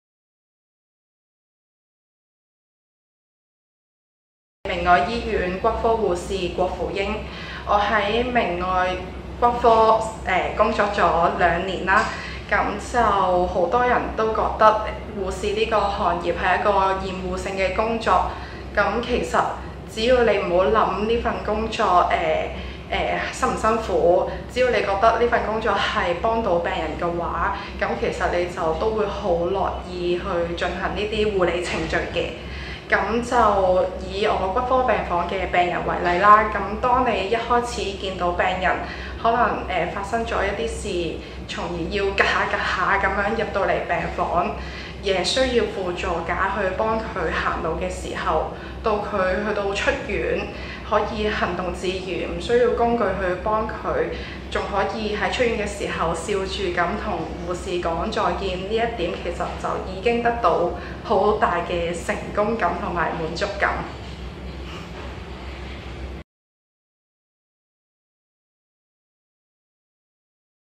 普遍認為護士經常要面對厭惡性工作 現職護士又有何看法呢？ | 骨科病房工作的註冊護士 | 求職增值
在骨科病房工作的註冊護士 (RN) 郭芙英，每天見盡骨折、車禍的病人。她表示，能在病人無助時伸出援手和看着病人笑着出院，便是工作最大的意義和滿足感。另外很多人認為護士經常要面對厭惡性工作，她又有何看法呢？

文章日期：2019年5月31日